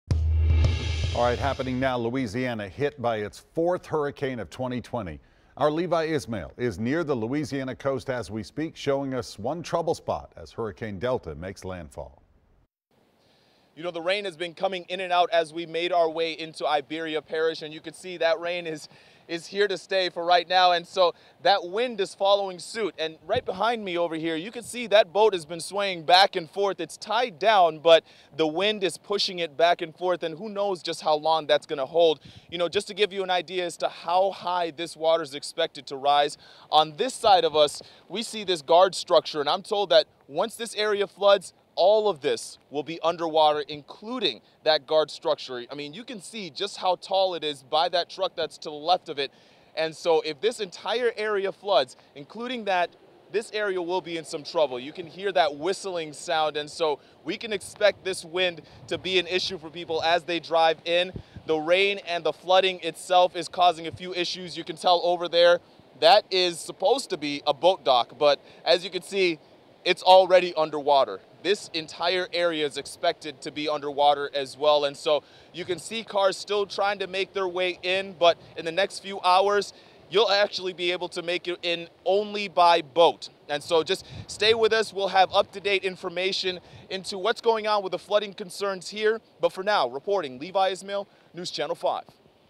Flooding begins in Louisiana ahead of Hurricane Delta
Hurrican Delta made landfall Friday bringing rain, flooding and damaging winds.